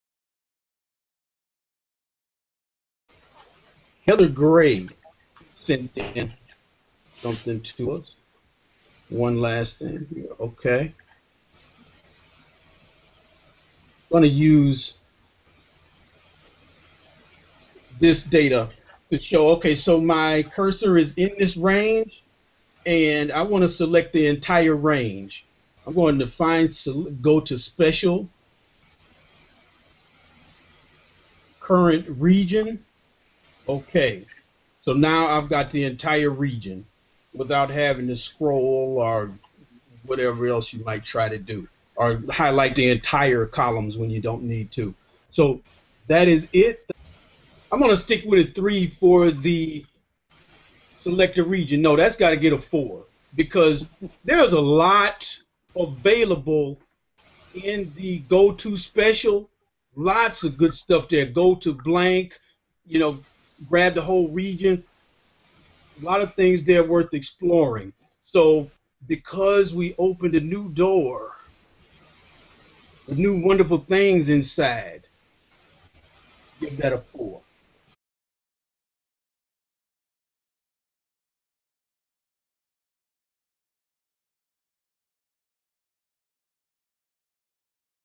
How to Select a Range in Excel - Excel Tips and Tricks
Heather Gray sends in this tip on how to quickly select a large range without needing to scroll to find the bottom rows.  Heather uses Go To Special.